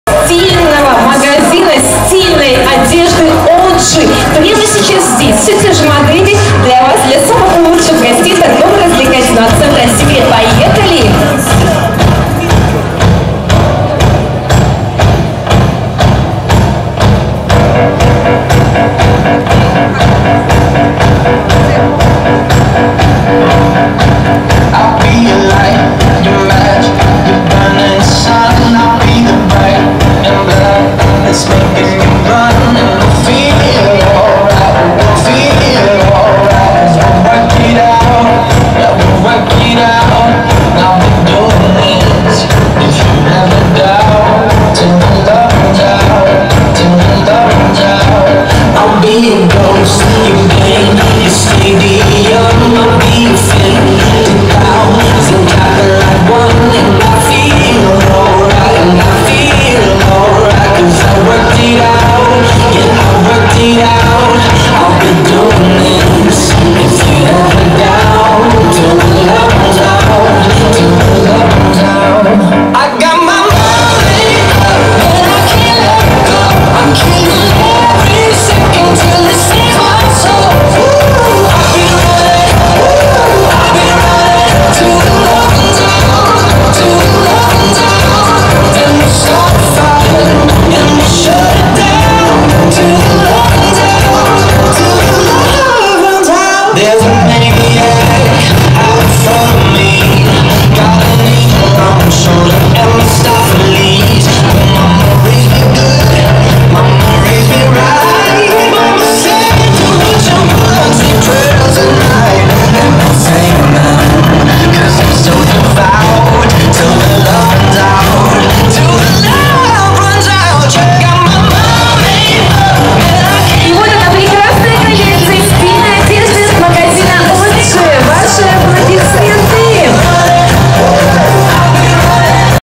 Модный показ коллекции Oodji. Агентство SigmaScouting Гомель // The collection of Oodji
ТРЦ"СЕКРЕТ". Модели агенства SigmaScouting Гомель приняли участие в показе коллекции Oodji.